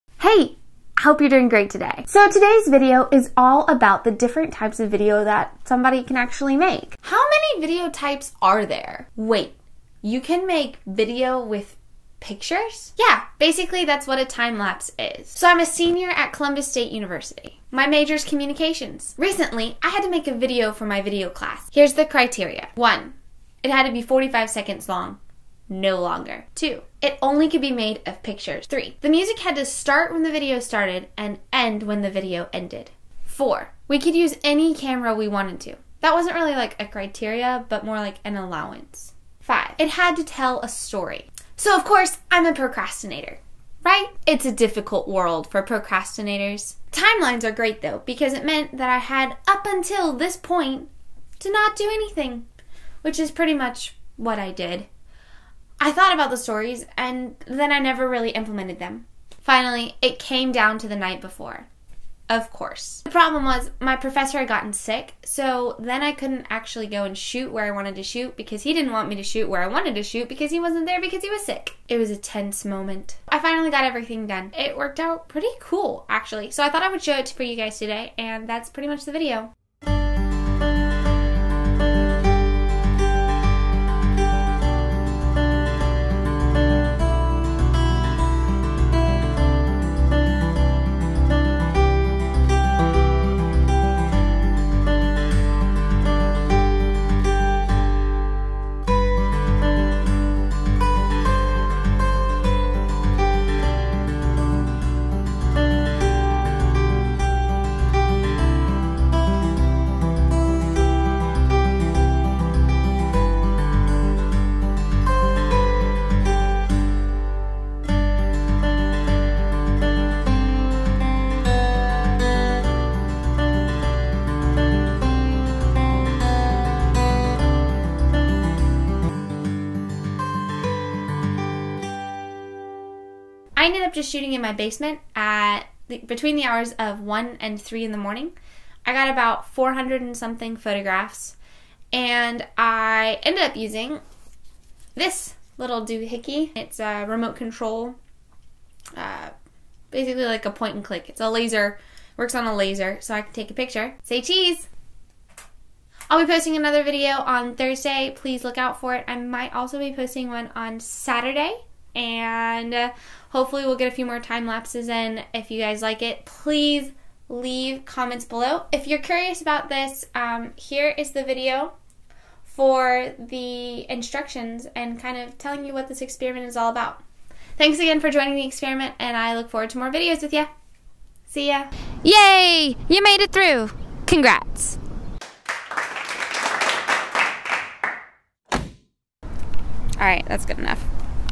ONF: TimeLapse - Creativity
To find out more about this experiment, please visit the wordpress!

I made a Timelapse of sorts recently, for my Video Production II class (taught by the awesome Chris R, who just so happens to be a fellow YouTuber as well - check him out at Electronic Truth TV
Anyways, let me know what you think! Also, props to anyone who can tell me where I got my favorite M mug. :)